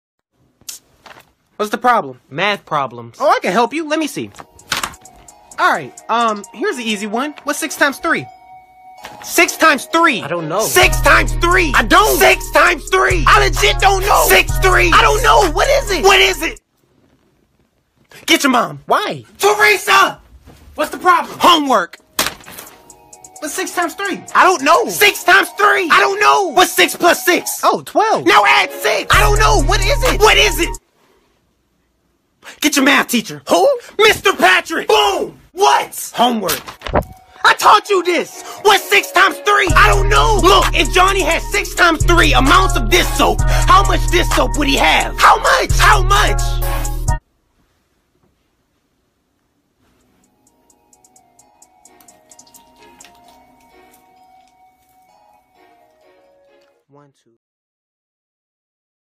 what's 6x3? [Percy Jackson animatic]
Characters from : Percy Jackson/Heroes of Olympus novel series by Rick Riordan

I know Annabeth couldn't be this stupid but I thought it would be fun if she joins the video XD
This is my first time making animatic. Hope you like it ^_^